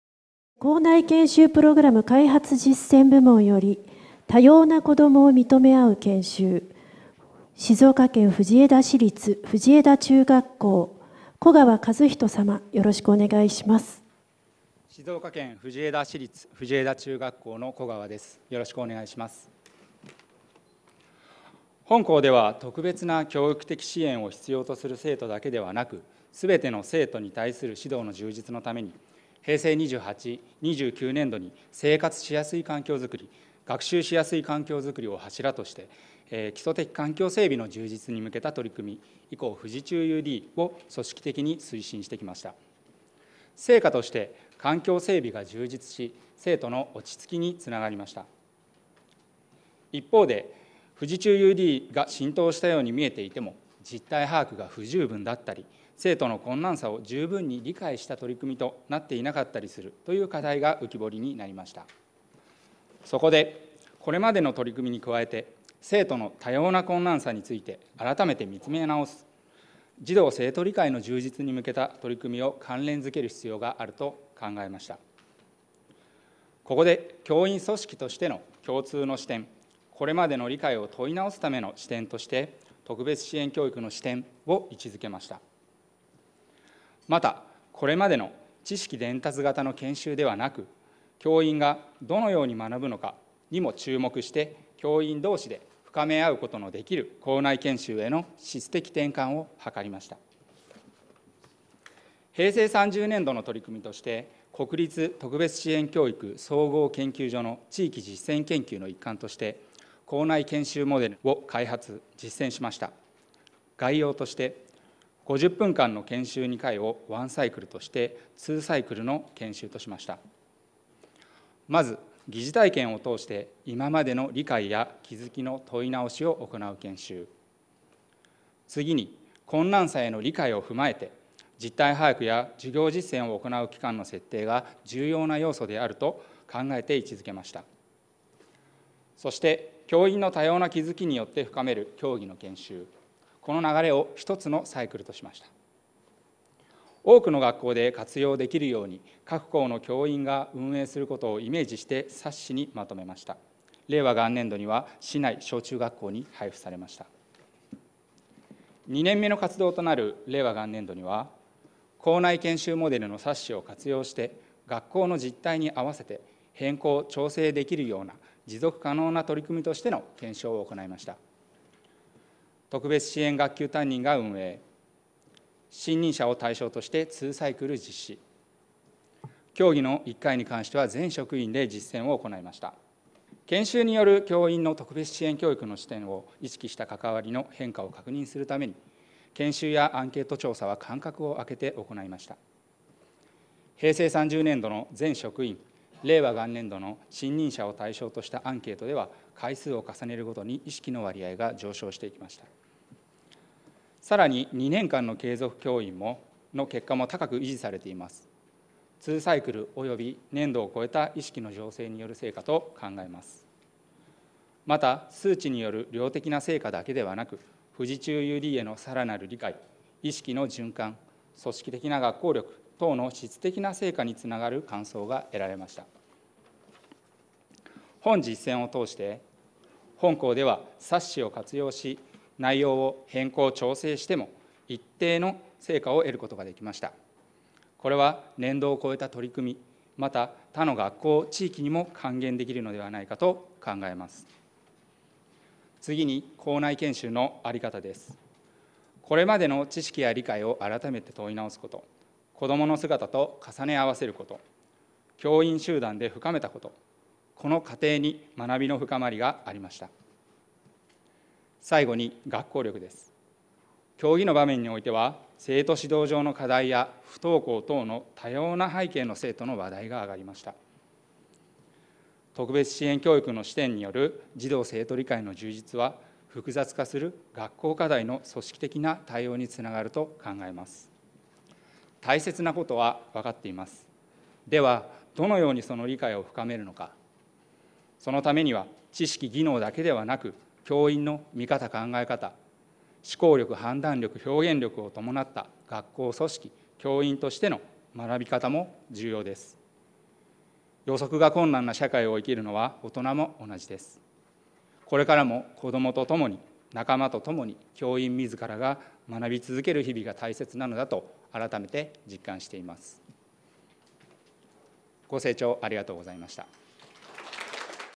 多様な子供を認め合う研修 ～特別支援教育の視点で高める学校力～（静岡県藤枝市立藤枝中学校 古川和史氏）：第3回NITS大賞活動発表 2020/02/02
教職員支援機構（NITS・ニッツ）では、学校をとりまく課題の解決に取り組んだ実践例を広く募集し、表彰・公開することにより、優れた実践例を普及していく表彰事業「NITS大賞」を実施しています。

令和元年度の第3回NITS大賞には、322点の応募があり、優秀賞10点・審査委員特別賞5点が選出され、令和2年2月2日（ニッツの日）に活動発表会を行いました。

本動画は、優秀賞に選出された 静岡県藤枝市立藤枝中学校 古川和史氏 が行った活動発表です。